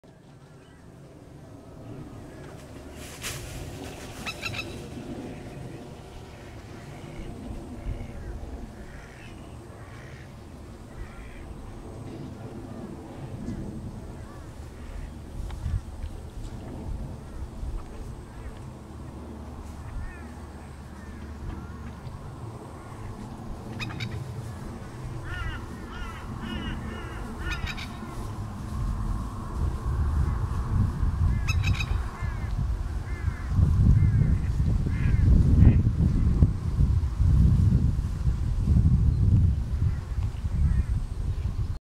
Pelican feeding near my house.
Not sure it would find much but it comes by once in a while.